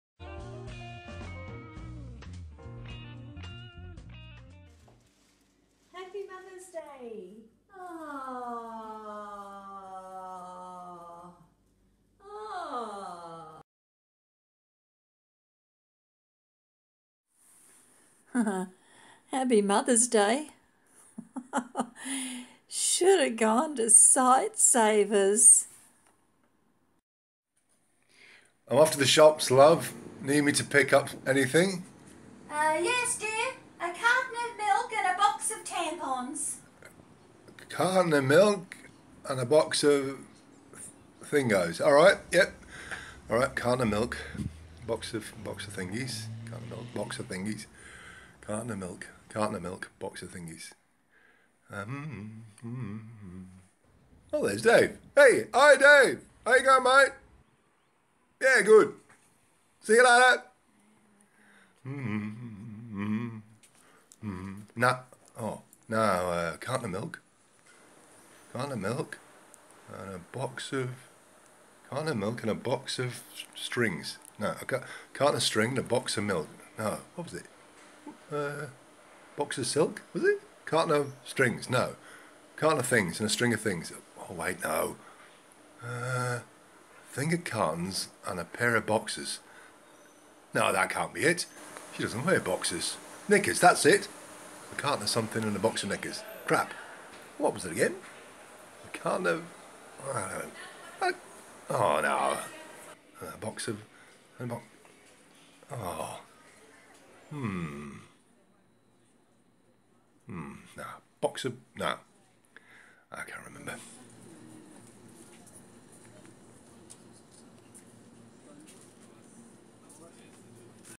DOMESTIC FAILS
Send your hubby shopping, it'll save you time they say.  Unless you're a hubby like this